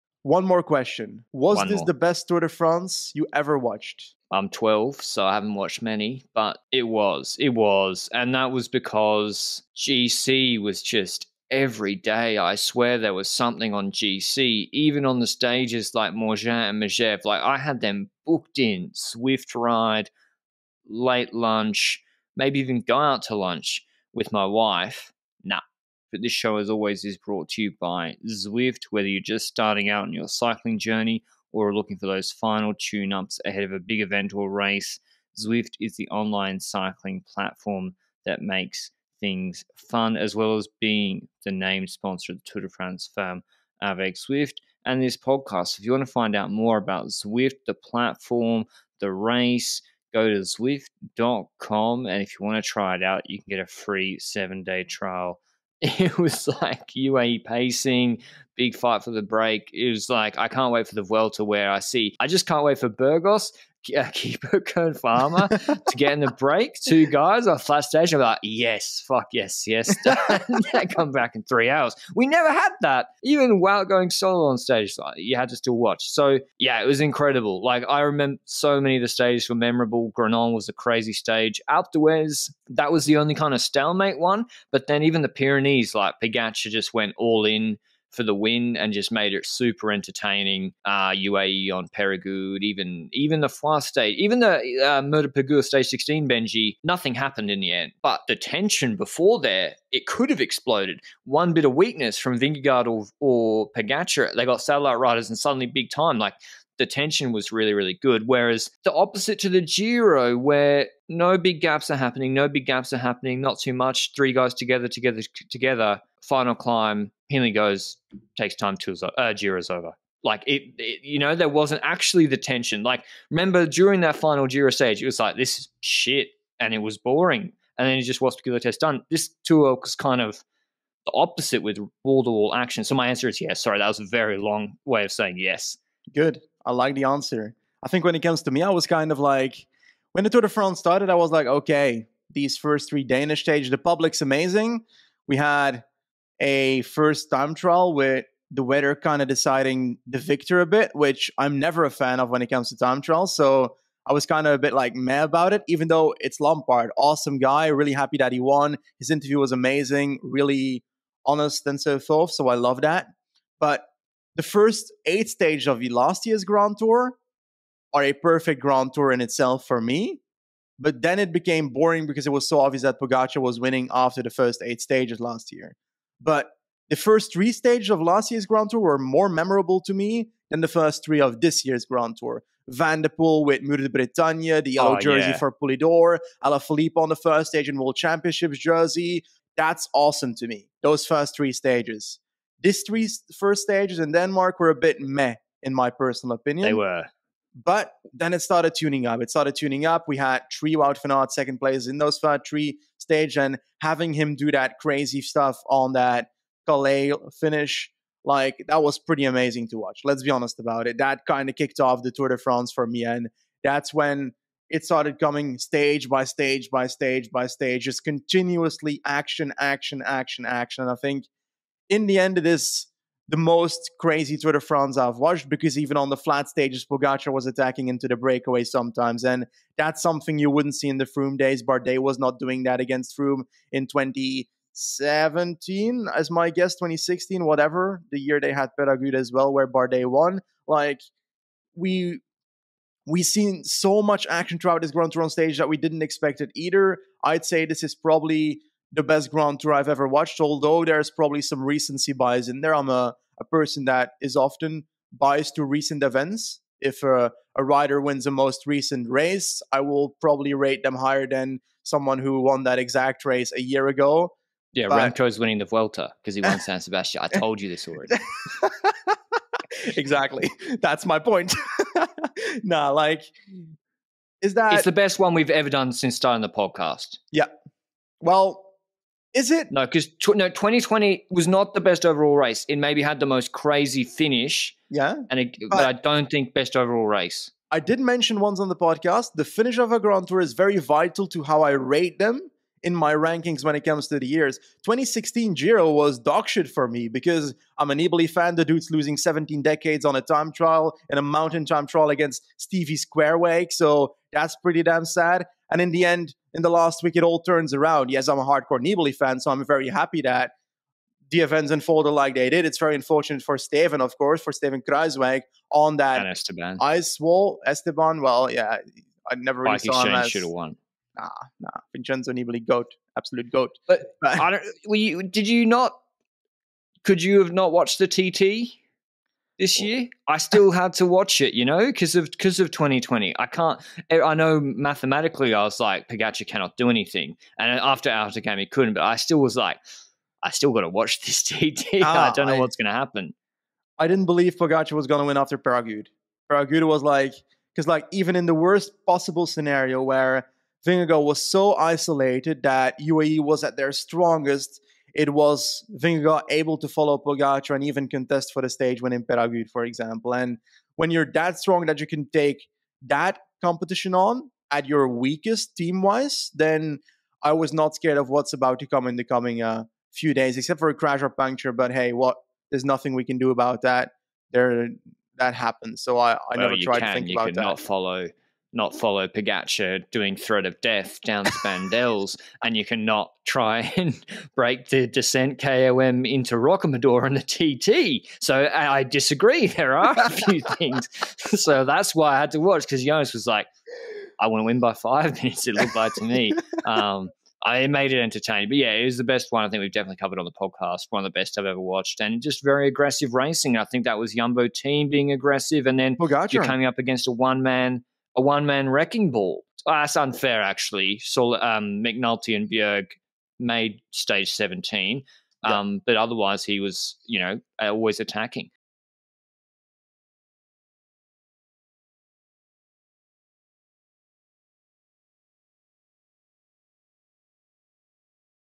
Was this the Greatest Tour de France of All Time? | LR & Benji Discuss | Lanterne Rouge x Zwift
Now that the dust has settled on the Tour de France 2022, LR and Benji discuss if it was the greatest edition they've ever watched.

The Lanterne Rouge Cycling Podcast is presented by Zwift.

Photos | Cor Vos (unless stated otherwise)